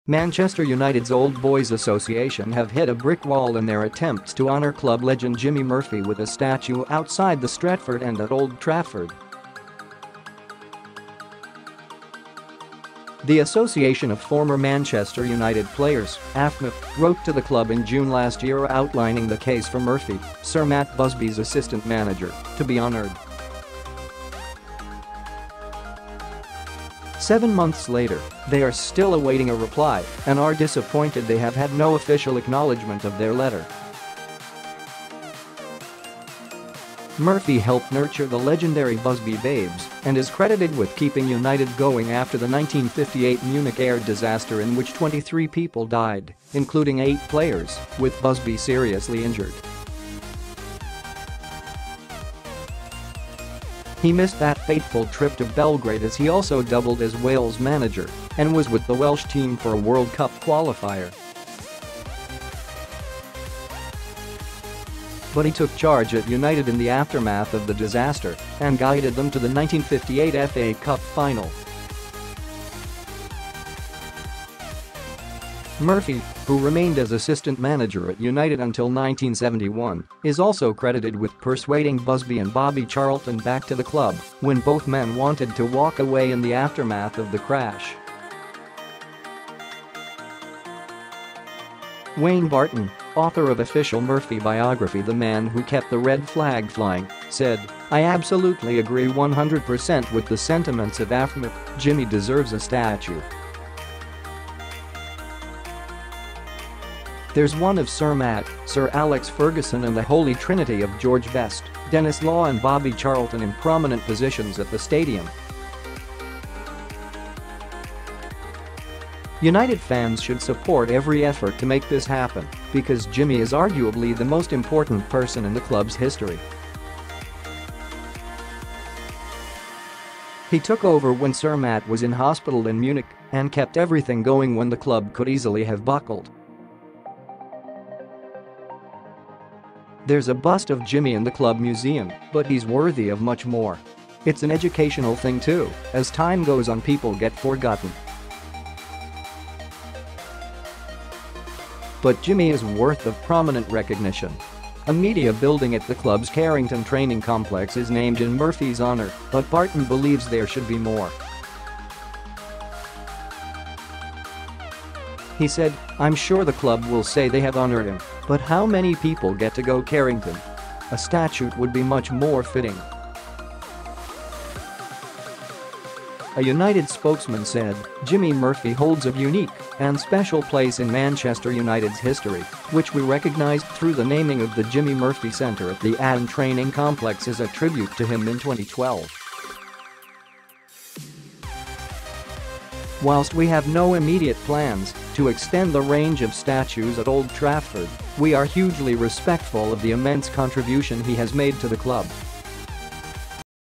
Man Utd old boys' dismay at club's silence over Jimmy Murphy statue idea
Manchester United's old boys association have hit a brick wall in their attempts to honour club legend Jimmy Murphy with a statue outside the Stretford End at Old Trafford.   The Association of Former Manchester United Players (AFMUP) wrote to the club in June last year outlining the case for Murphy, Sir Matt Busby’s assistant manager, to be honoured.   Seven months later, they are still awaiting a reply — and are disappointed they have had no official acknowledgement of their letter.  Murphy helped nurture the legendary Busby Babes and is credited with keeping United going after the 1958 Munich air disaster in which 23 ­people died – including eight players – with Busby seriously injured.   He missed that fateful trip to Belgrade as he also doubled as Wales manager and was with the Welsh team for a World Cup qualifier. But he took charge at United in the aftermath of the disaster and guided them to the 1958 FA Cup Final.  Murphy, who remained as assistant manager at United until 1971, is also credited with persuading Busby and Bobby Charlton back to the club when both men wanted to walk away in the aftermath of the crash.  Wayne Barton, author of official Murphy biography The Man Who Kept The Red Flag Flying, said: “I absolutely agree 100 per cent with the sentiments of AFMUP – Jimmy deserves a statue. There’s one of Sir Matt, Sir Alex Ferguson and the ‘holy trinity’ of George Best, Denis Law and Bobby Charlton in prominent positions at the stadium.   “United fans should support every effort to make this happen because Jimmy is arguably the most ­important person in the club’s history. He took over when Sir Matt was in hospital in Munich and kept everything going when the club could easily have buckled.  “There’s a bust of Jimmy in the club museum but he’s worthy of much more. It’s an educational thing too — as time goes on people get forgotten. But Jimmy is worth of prominent recognition.’’  A media building at the club’s Carrington training complex is named in Murphy’s honour – but Barton believes there should be more.   He said: “I’m sure the club will say they have honoured him but how many people get to go Carrington? A statute would be much more fitting.”  A United spokesman said: “Jimmy Murphy holds a unique and special place in Manchester United’s history, which we recognised through the naming of the Jimmy Murphy Centre at the Aon Training Complex as a tribute to him in 2012.   “Whilst we have no immediate plans to extend the range of statues at Old Trafford, we are hugely respectful of the immense contribution he has made to the club.”